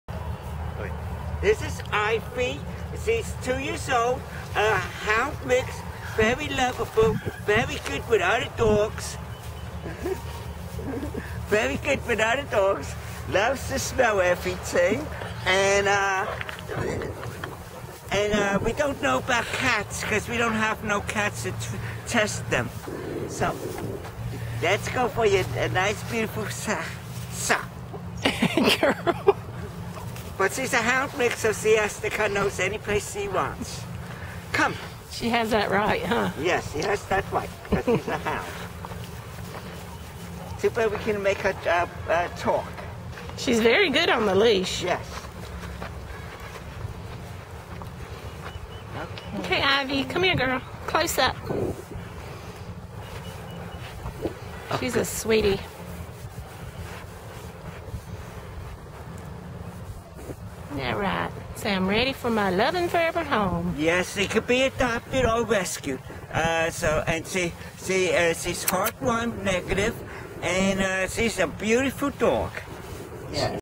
ID#0313, IVY, ready for adoption/rescue, Darlington County Humane SocietyDSCN59401-Desktop.mov
Darlington County Humane Society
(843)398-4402
I am ready for adoption or rescue!!!
ID#0313, IVY, female, 5-6 years old, Hound, HW NEG., Intake: 2-6-13
Sweet Ivy is gentle and calm. She is a very good leash walker, good with people, and other dogs. Cats?
Dialogue credited to Susan Blakeney:
A Face Cleaner!  Have you ever seen such a lovely, lovely beauty to keep you face freshly kissed?  Ivy has a lot of awesome pet skills and while kissing is surely one of her best attributes, she is also very huggable and loveable and you will find yourself wanting to soak her up like a big sponge!   A lower energy, unflappable lady,  Ivy has put many miles on her pads, had several litters of pups and is now ready to settle in and simply focus on loving her final person.   Well mannered and as gentle as they come,  Ivy will quietly sit right by your side as her tail taps with pure contentment against your side.   I love this dog and you will as well!!    53 lbs; hw -; 2/6/13; hw -  Note:  Ivy is terrific with other dogs. Terrific!